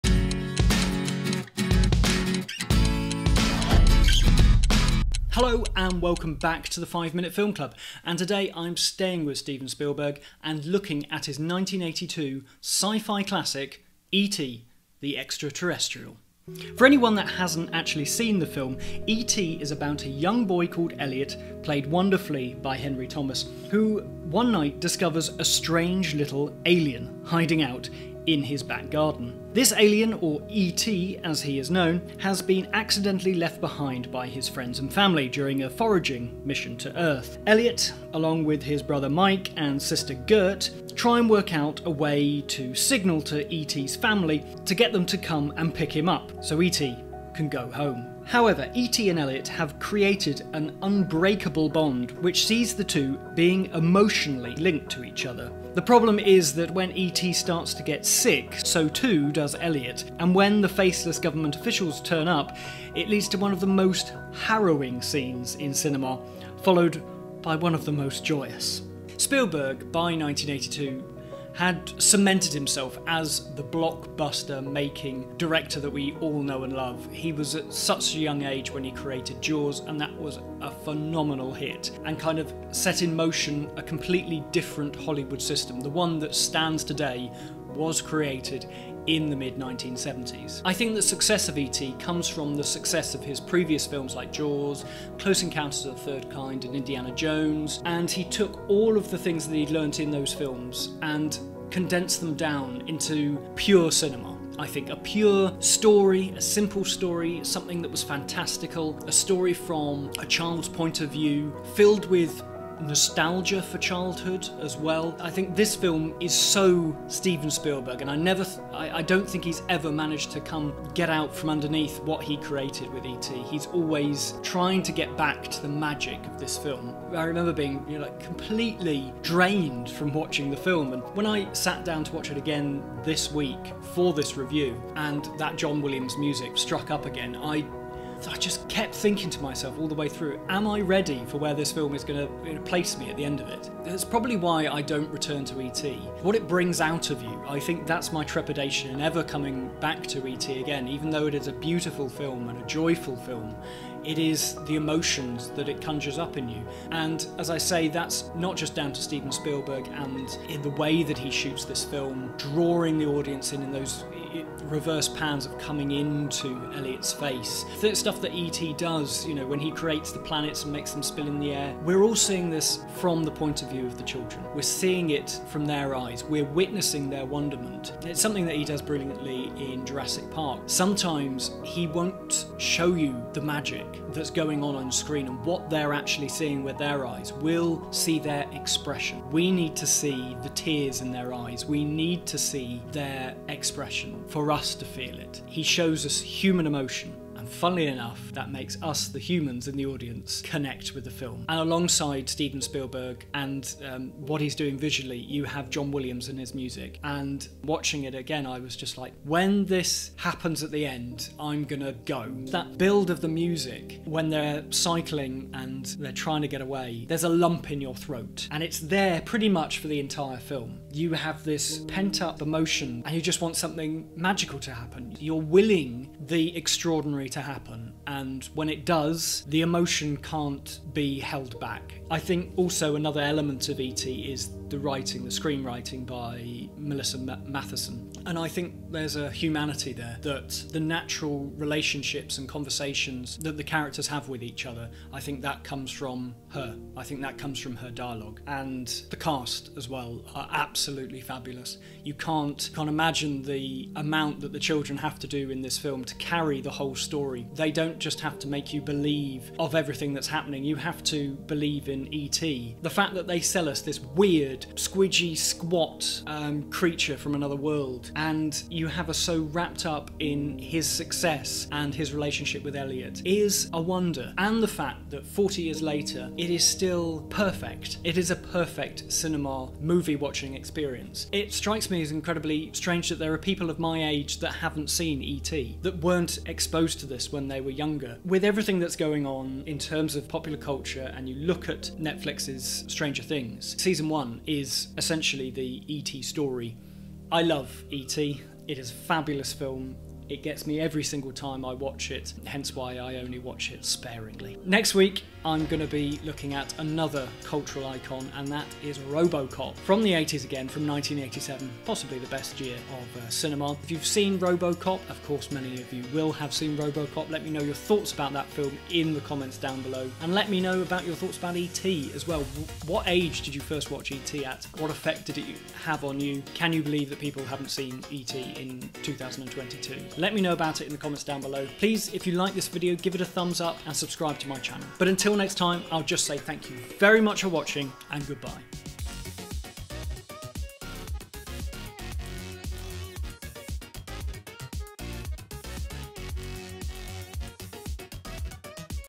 E.T. The Extraterrestrial (1982) Movie Review | Science Fiction and Fantasy | 501 Must See Movies
MOVIE SUMMARY

HE IS AFRAID. HE IS ALONE. HE IS THREE MILLION LIGHT YEARS FROM HOME.

After a gentle alien becomes stranded on Earth, the being is discovered and befriended by a young boy named Elliott. Bringing the extraterrestrial into his suburban California house, Elliott introduces E.T., as the alien is dubbed, to his brother and his little sister, Gertie, and the children decide to keep its existence a secret. Soon, however, E.T. falls ill, resulting in government intervention and a dire situation for both Elliott and the alien.

Director
Screenplay
Cast

GEAR
iPhone XS
Samson Q2U
Neewer Mini LED Video Lights